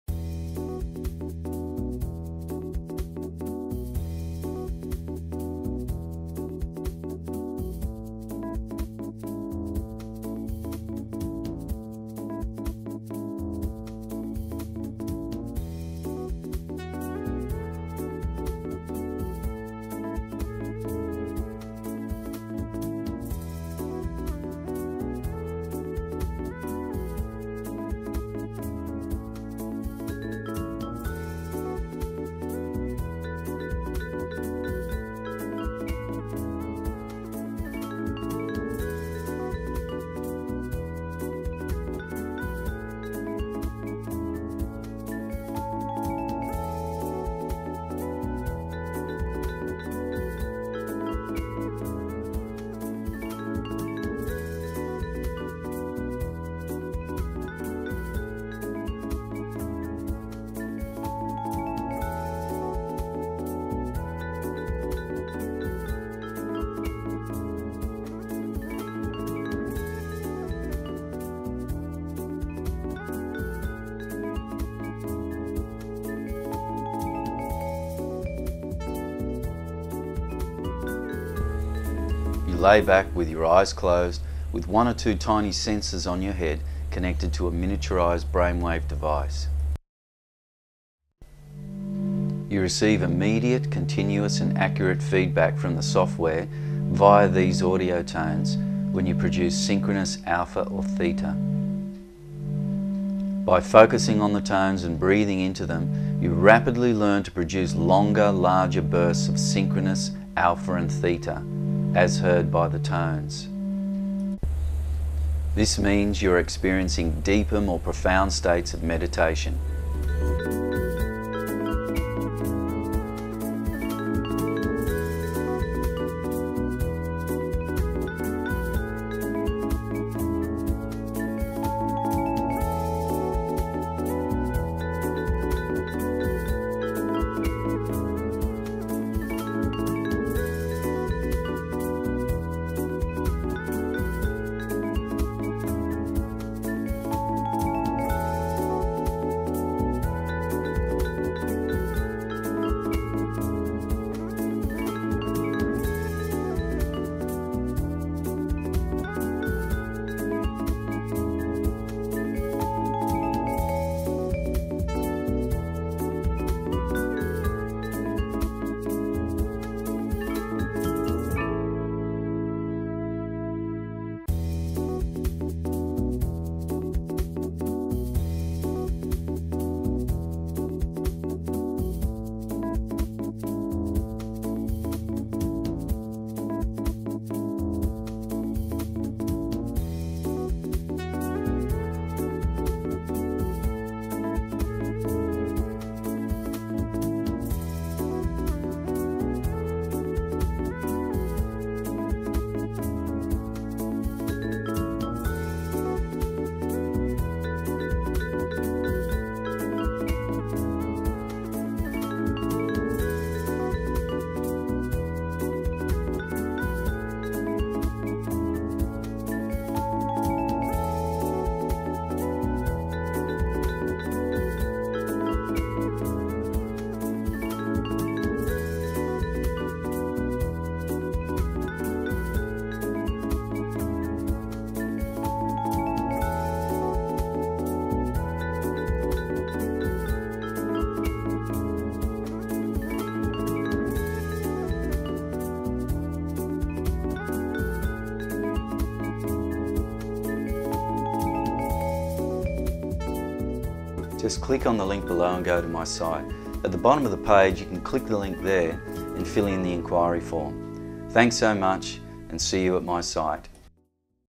Alpha Theta Neurofeedback
The most powerful way to learn or deepen meditation. Would you like to see into your brain and know when you are generating synchronous alpha or theta brainwaves like Zen masters? Now you can with brainwave biofeedback, be guided by your own brainwaves.